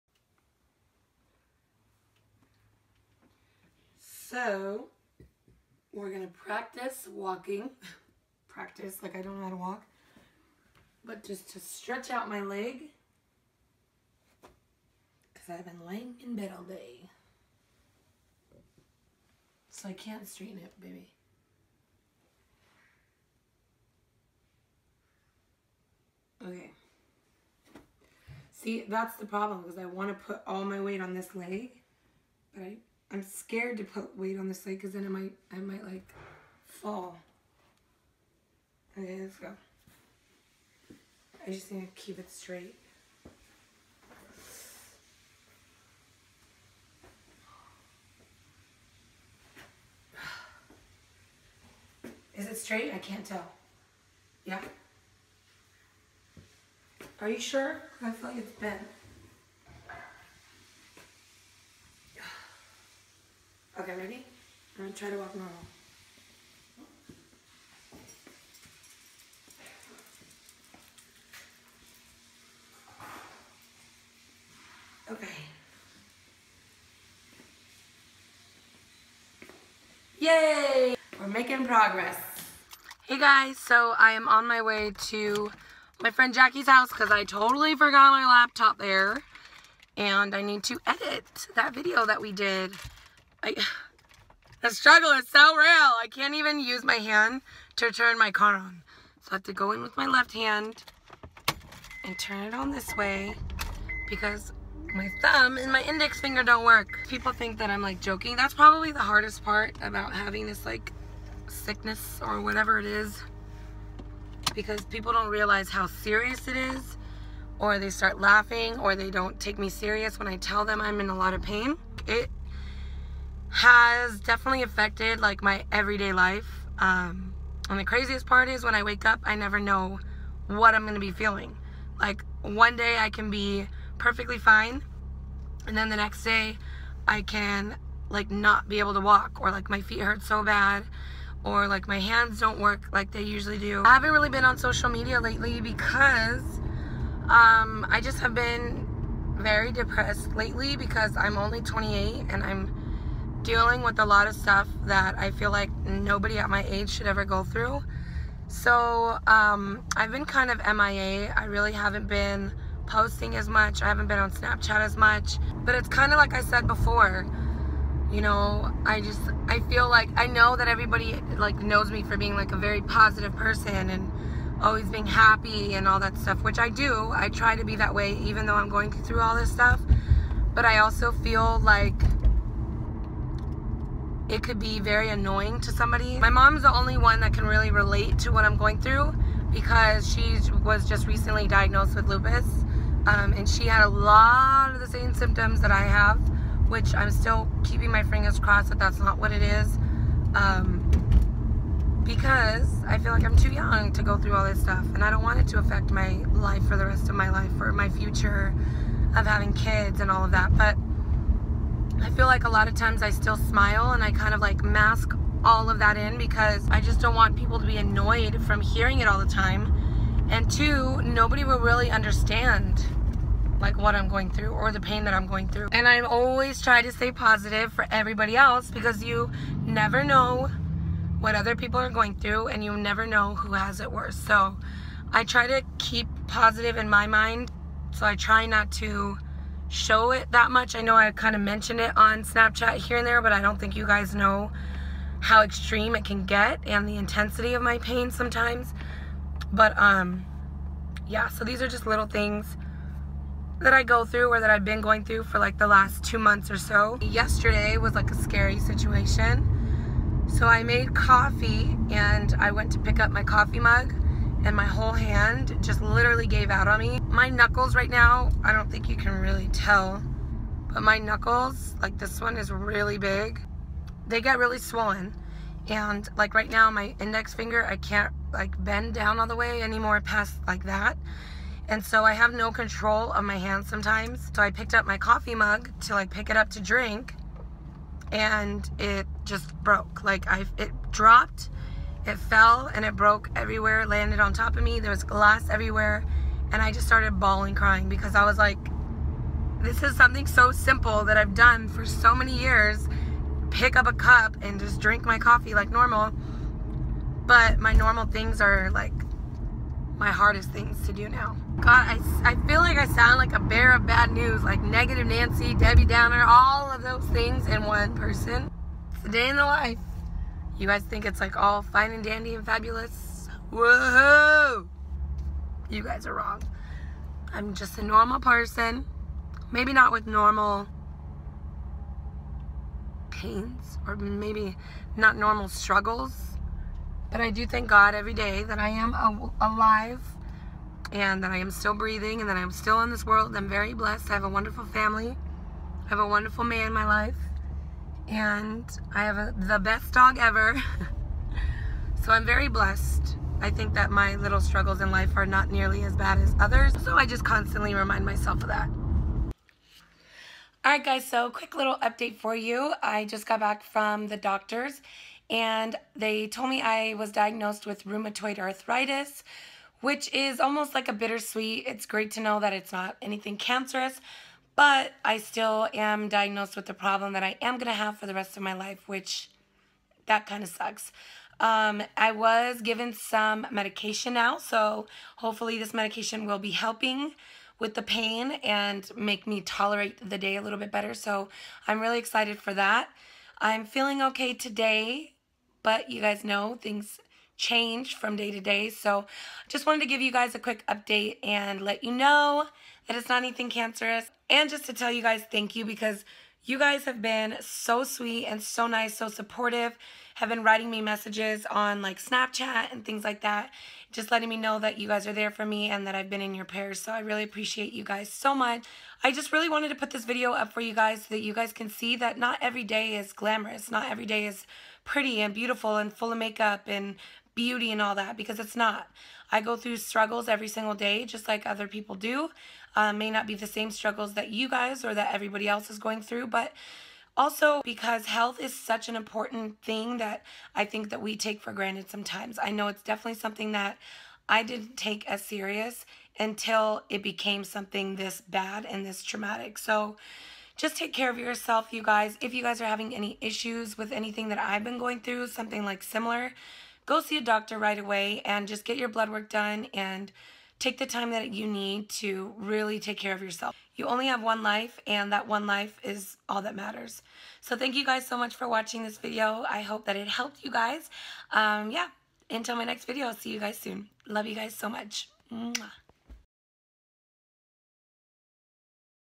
BEING SICK | GETTING DIAGNOSED
I know this is not your typical "Lycette" kind of video. But if you want to see my REAL life, and things I struggle with day to day, then this is it. Its unfortunate to have to go through something like this at my age, and its been a pretty rough ride so far. I was recently diagnosed with Rheumatoid Arthritis. Rheumatoid arthritis is a form of arthritis that attacks the bodies joints and nerve system and can be mild to severe depending on the patient. Keeping my fingers crossed it will only get better from here.

Thank you guys so much for all your love on Snapchat and IG. I Appreciate each and every one of you!!

Lycette